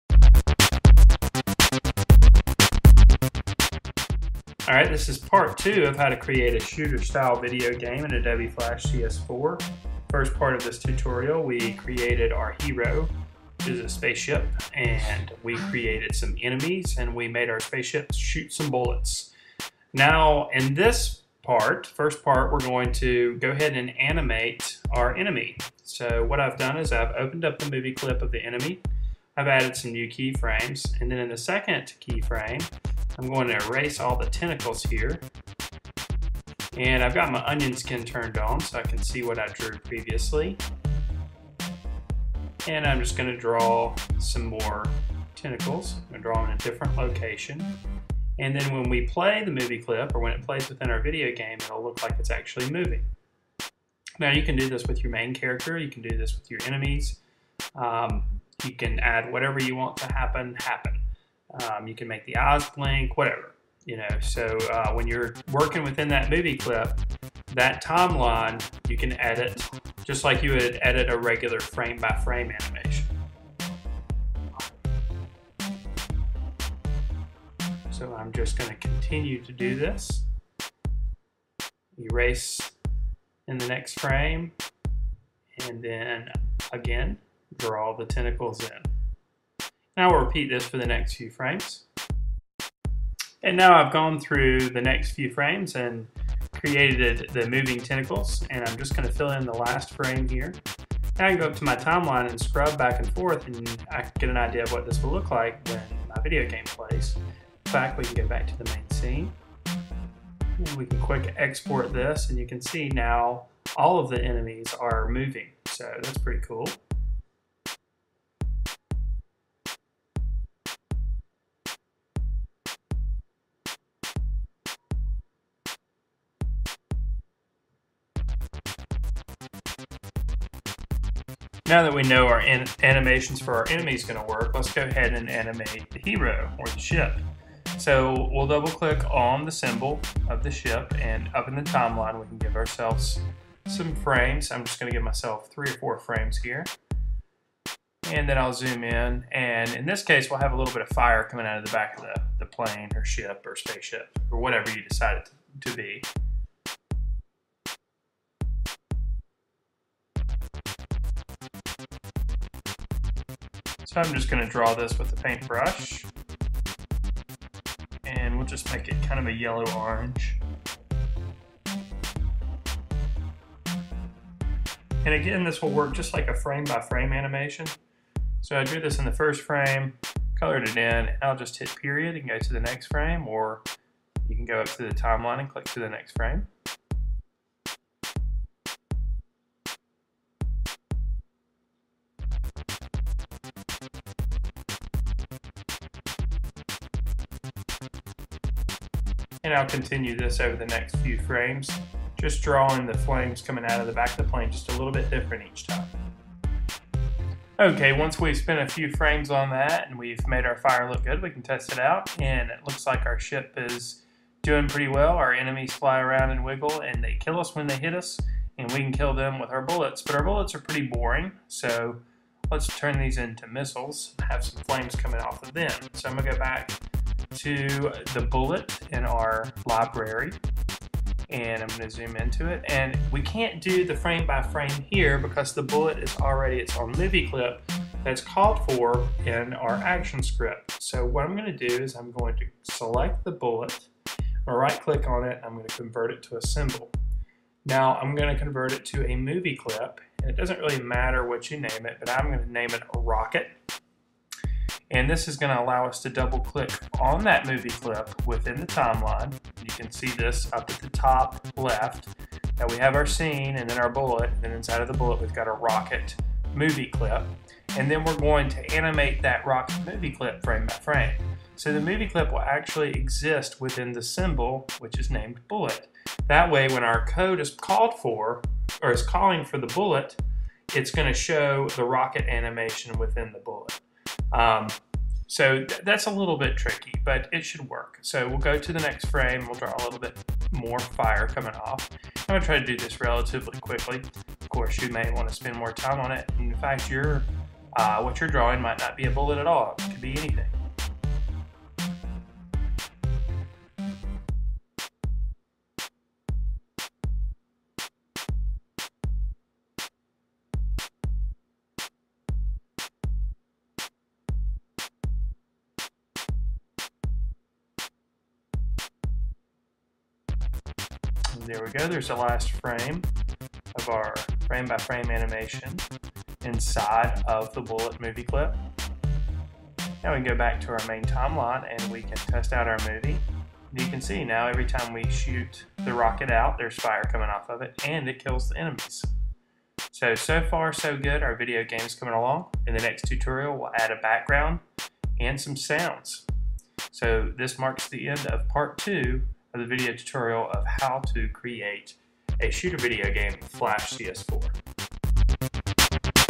How to create a shooter style video game in Adobe Flash- Part 2
Codes for this tutorial can be found as well as step by step instructions.  How to create a shooter style video game in Flash third person-part 2